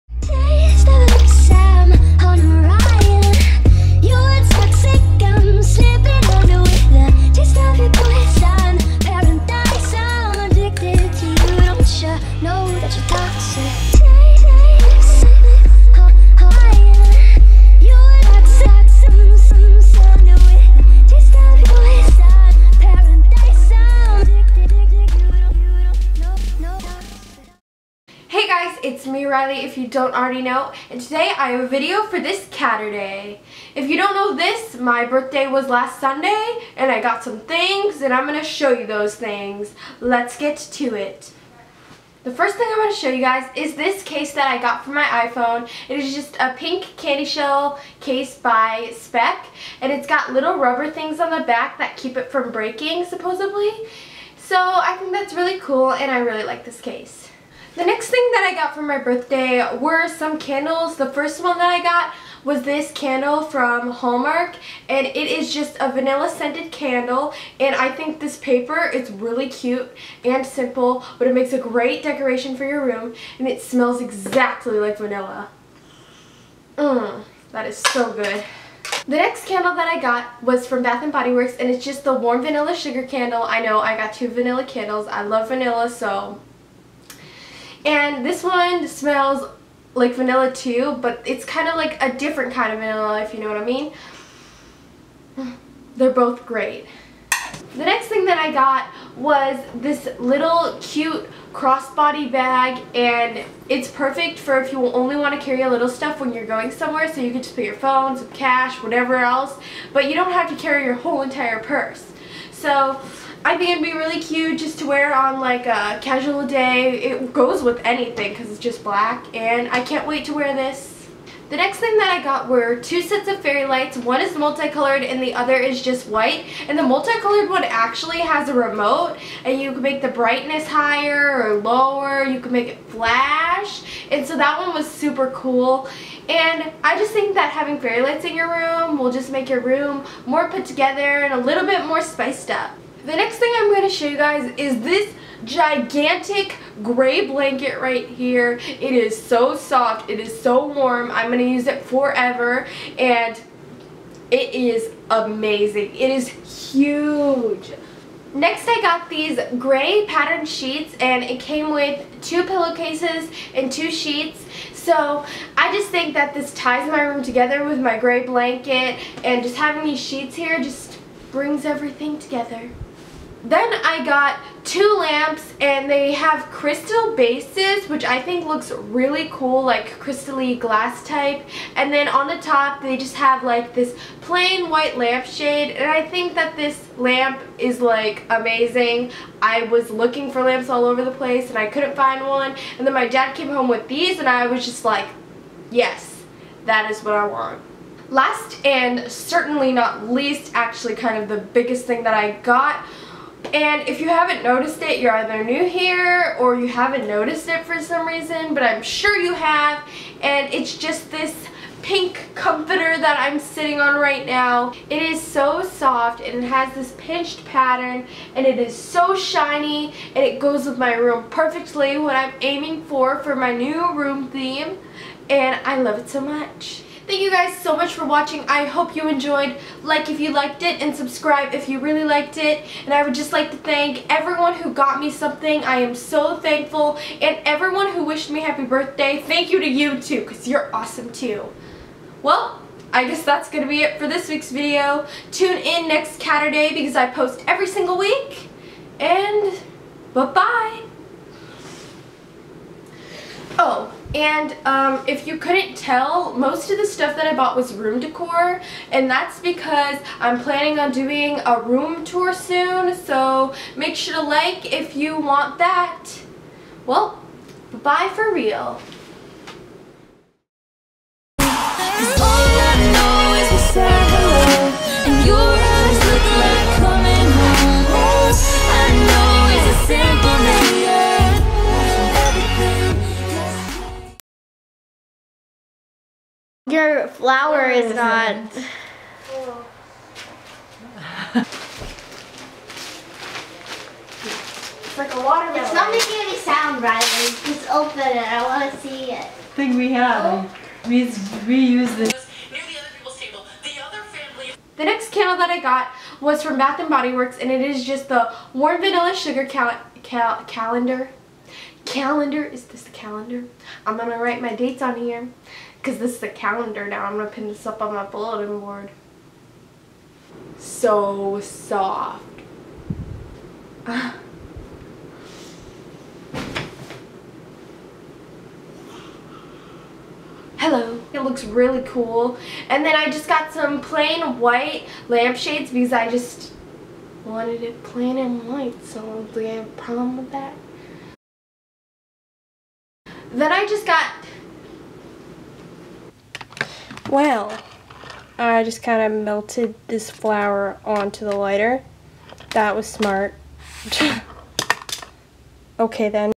Birthday Haul 2016 | Riley Meow
In this video I show you what I got for my birthday.  Hope you enjoyed!  I really liked making this one!  LIKE if you liked it.  COMMENT WHEN YOUR BIRTHDAY IS!